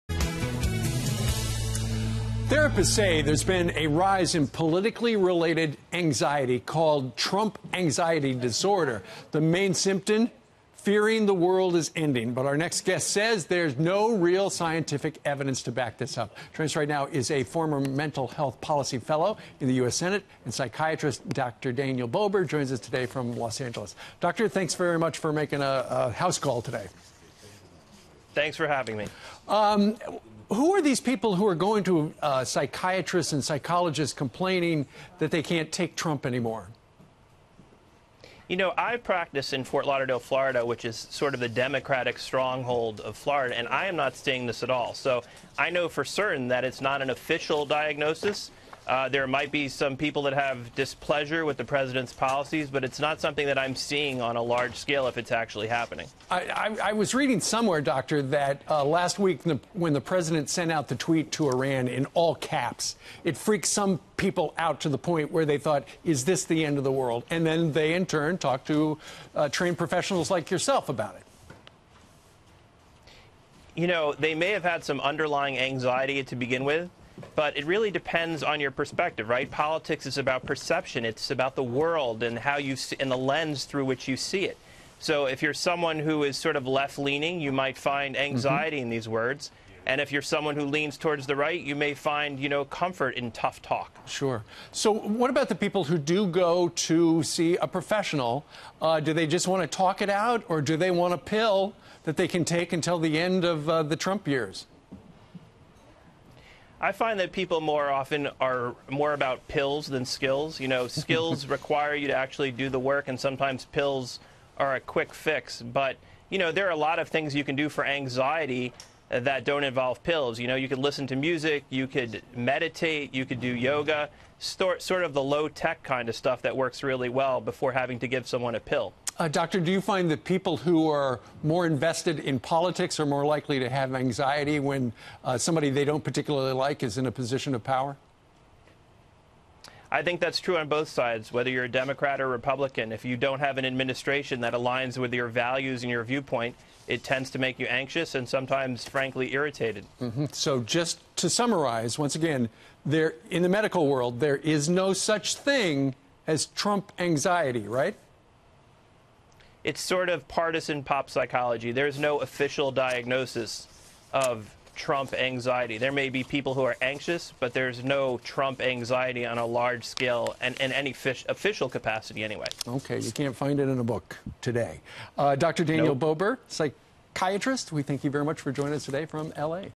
Psychiatrist Says No Evidence Of 'Trump Anxiety Disorder'
July 30, 2018 - Dr. Daniel Bober calls the diagnosis trend 'partisan pop psychology.'